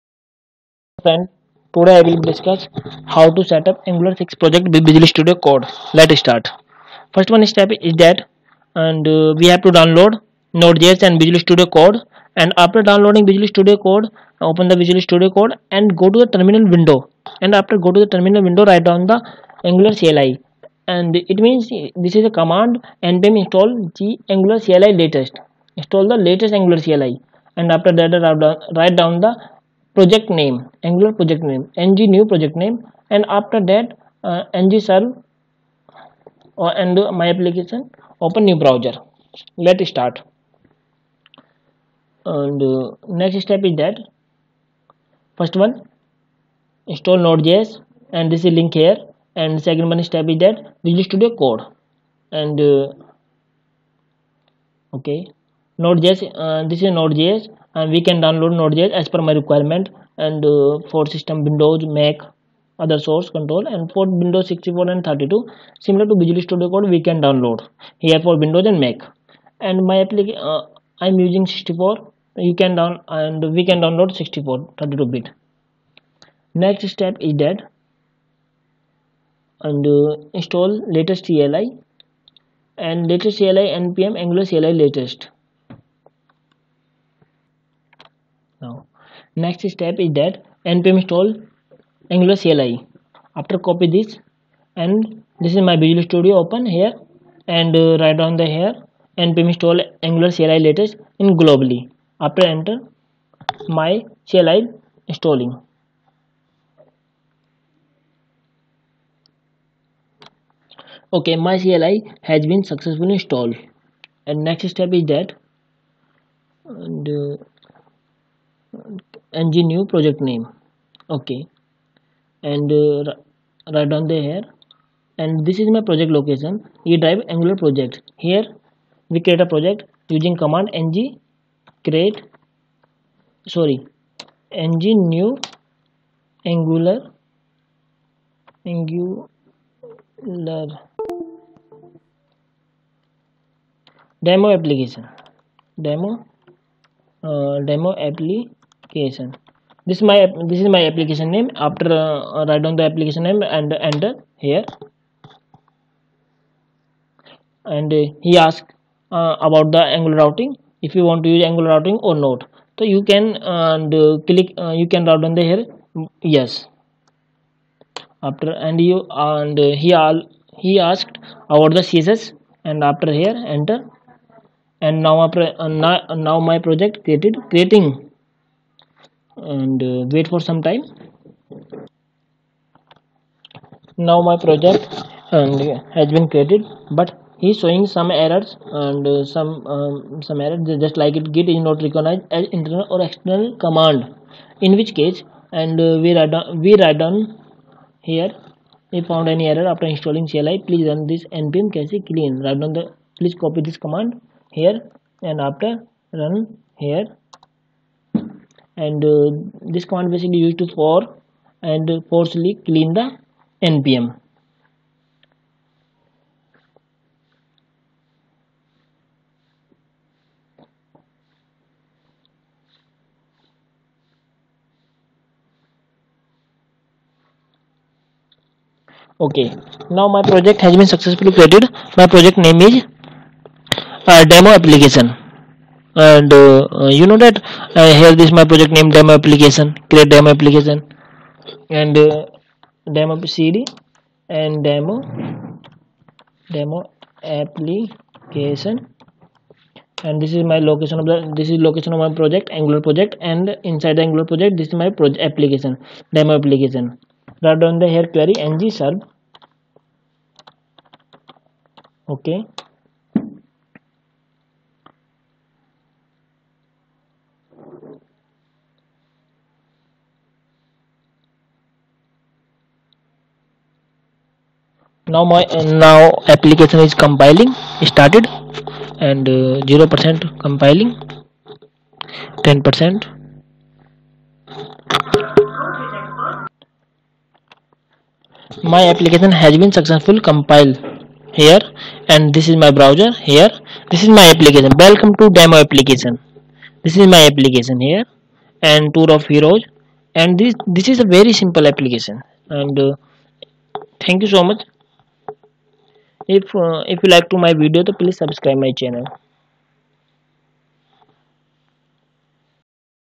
Angular 6,7,8,9 Setup New Project With Visual Studio Code
This video main purpose create a new angular project with visual studio code